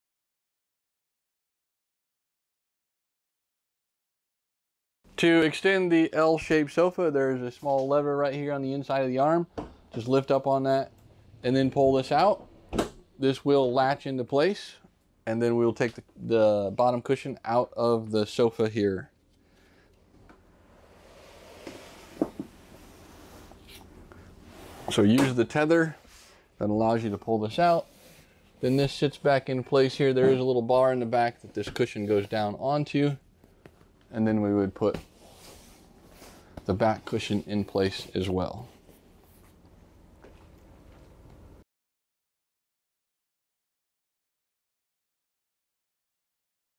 Entegra Coach L Shaped Sofa Delivery Video
This Delivery Video is presented to you by PJ Clanton and our Technical Training Department here at Entegra Coach. We hope this video helps assist you with the delivery of your new coach, and provides you with basic knowledge of the components shown in these videos.